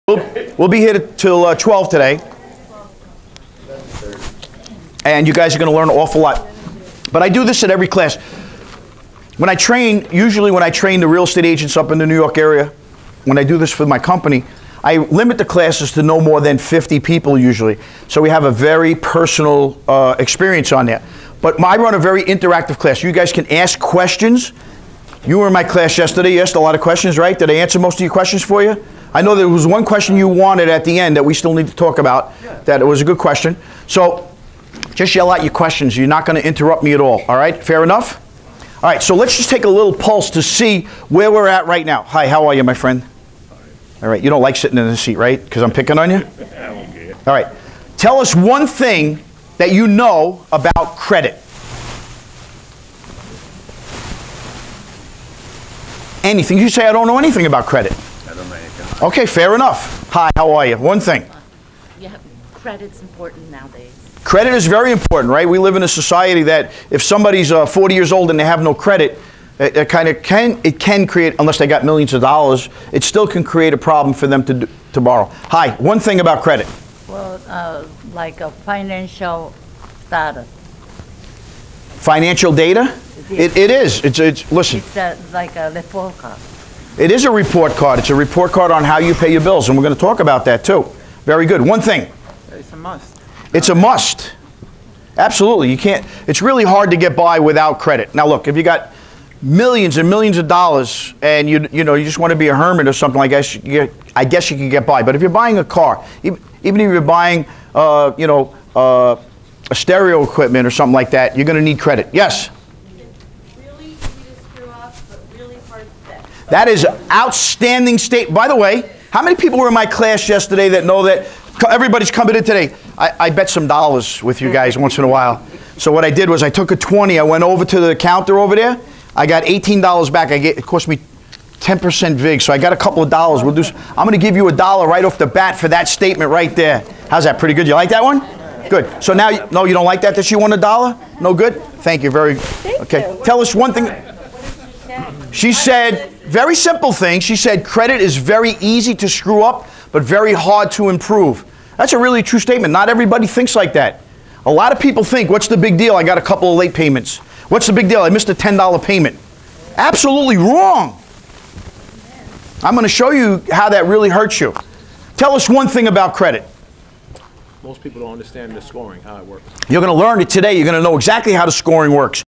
Doug asks students at start of class What they know about Credit
At the Start of most of Doug Vairo's classes he will ask students what they know about the subject being taught. Listen to some of the answers that Real Estate Agents give in the CE class at Triple Play.